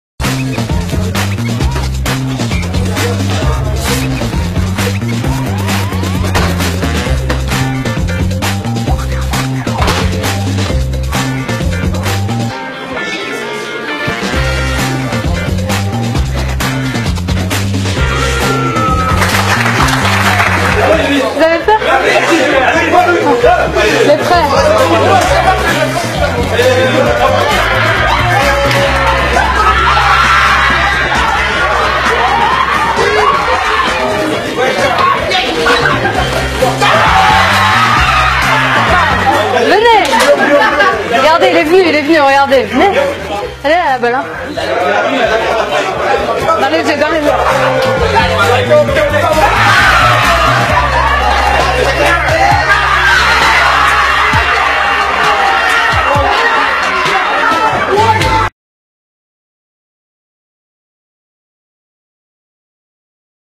FREESTYLER DESTORYED PSG YOUTH TEAM 2017
PSG youth team getting destroyed by lady freestyler in the changing room.